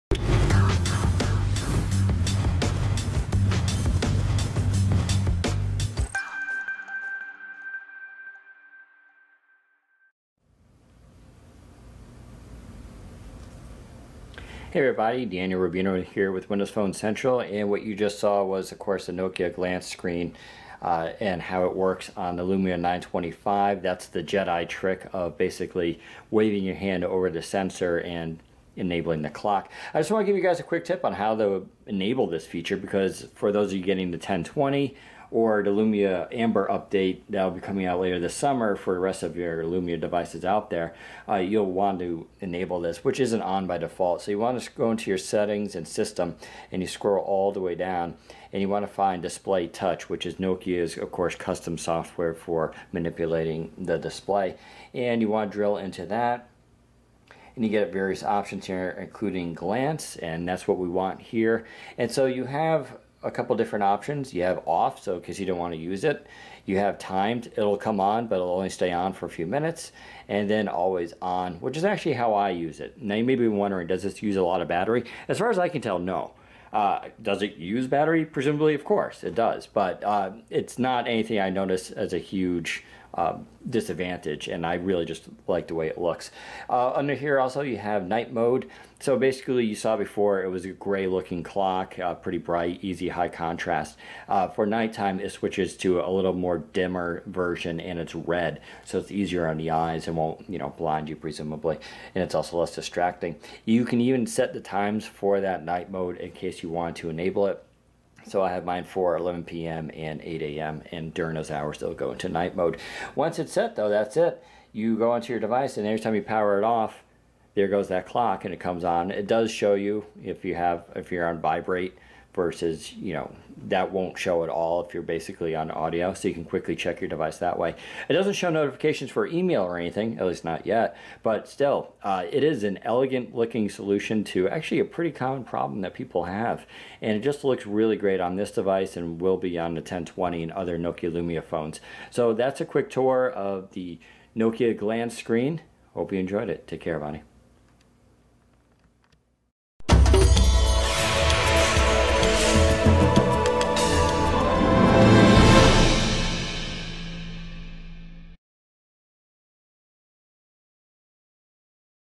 Nokia Glance Screen how to for Lumia Windows Phones and the Lumia 925
Nokia has a new feature for their Lumia phones called 'Glance' that puts an always-on clock on your display. We show you how to enable it and how to use it.